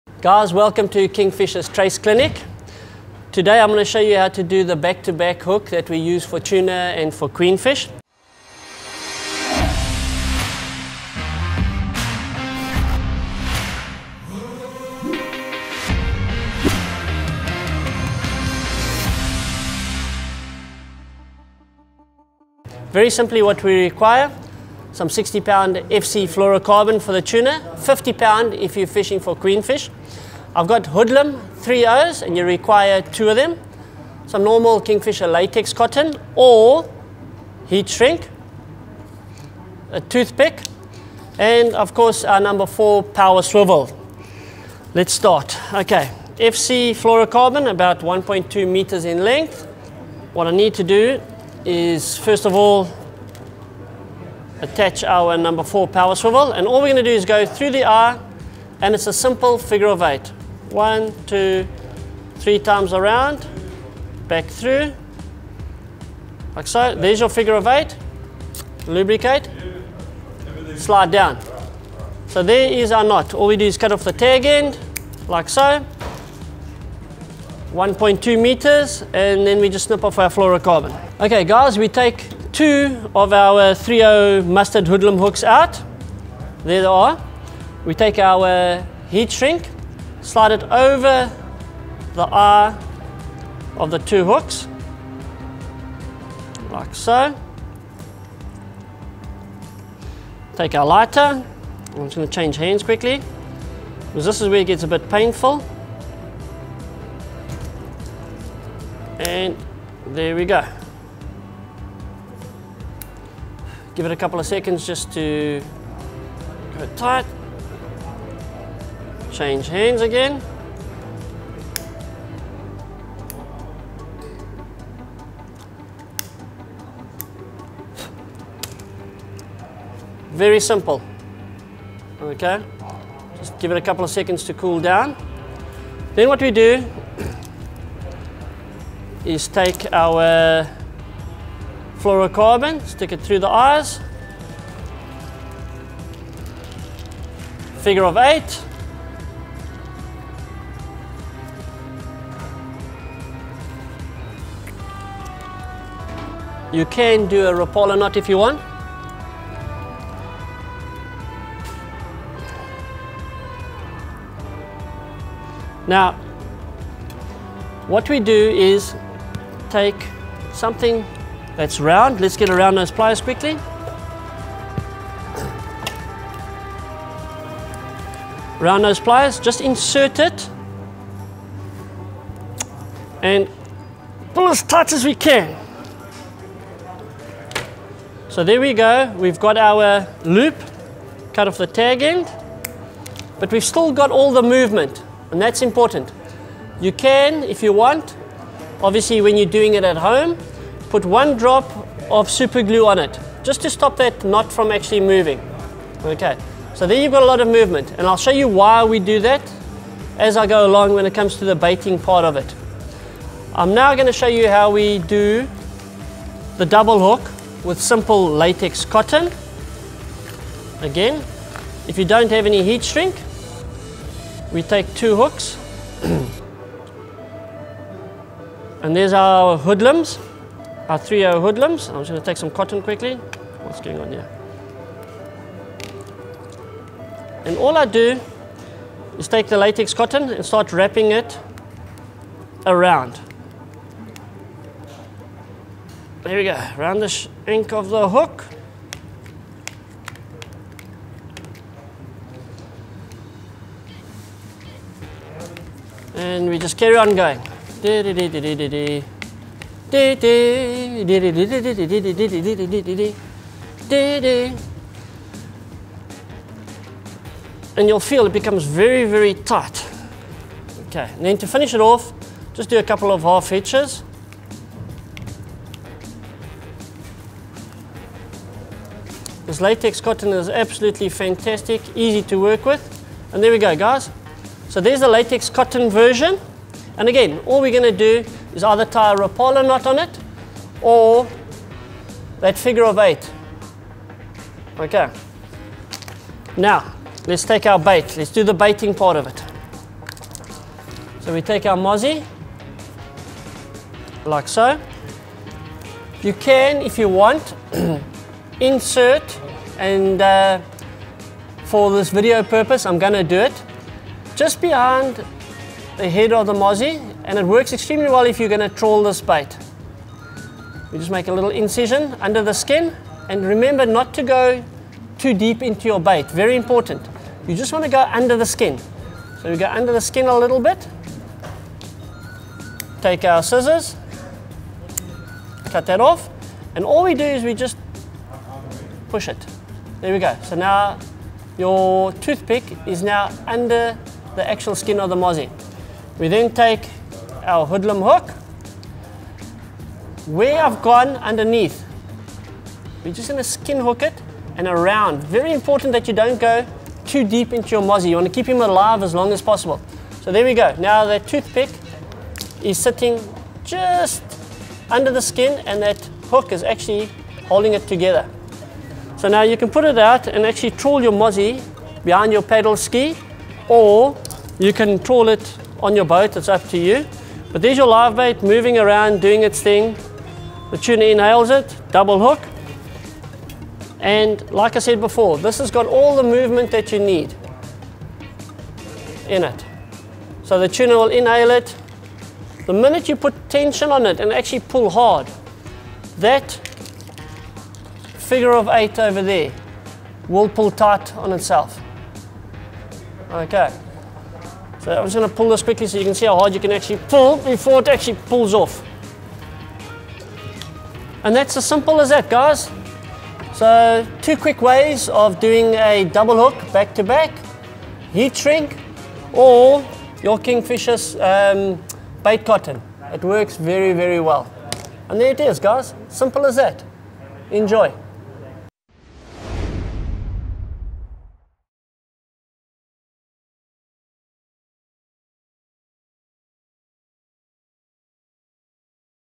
Traces | Tuna Part III
Yellowfin Tuna, aka Tunny. One of the most targeted pelagic specie in South African waters. And for good reason, they're absolute brutes to fight, one of the most sustainable species to take home for the pot (sushi rolling matt) and they are such highly aggressive feeders. In this IV part series of Tuna Traces, Ray demonstrates how to make your own trace to target the Tuna and the pros and cons of each trace.